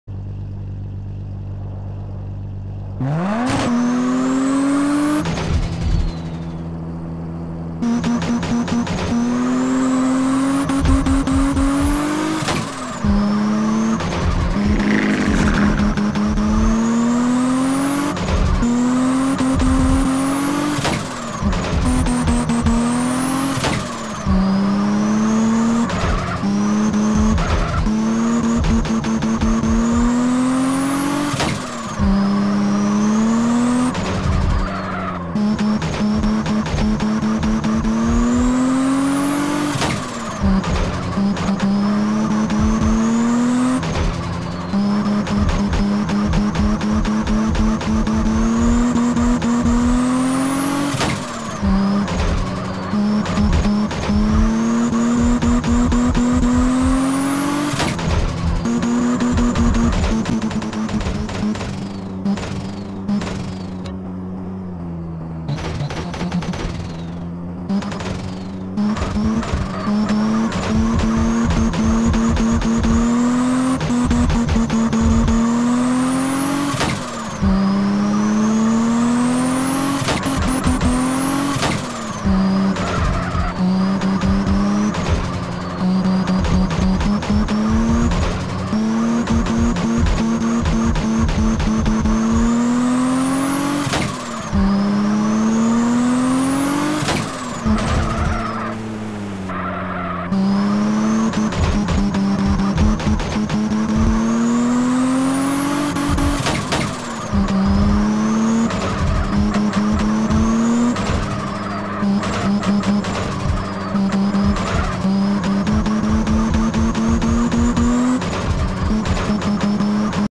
650 HP TURBO Mk 5 SUPRA Trial Mountain Drift
Hey guys am back with another video, am excited to be posting again, feels like it's been a while lol, enjoy :)

Special Thanks to: Hanazono Modding, rubenoliveira and AhmadSyahmi